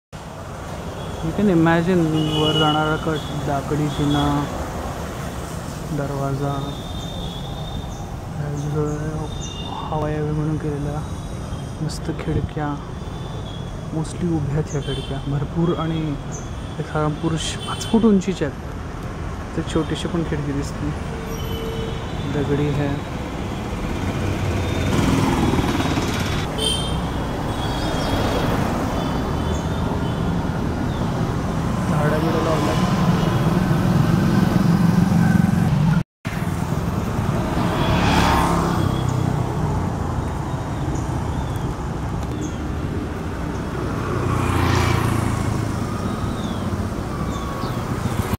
Old Mansions I Shivajinagar I Sangli I Solo Trip with Saurabh
स्थळ : जुन्या बैठ्या घरांच्या इमारती, उत्तर शिवाजीनगर, सांगली.

विकिमेडीया फौन्डेशनसाठी सांगली शहरातील ७५ पेक्षा जास्त वय असलेल्या
वास्तूंचे दस्तावेजीकरण स्वयंसेवी तत्त्वावर करण्याचा हा व्हिडियो एक भाग
आहे.
वर्ष २०२१

Venue: Old Mansions at Shivajinagar, Sangli

This video is a part of documenting the 75+age old structures from
Sangli City (Maharashtra) on volunteer basis.
Year 2021.

सौरभ I Saurabh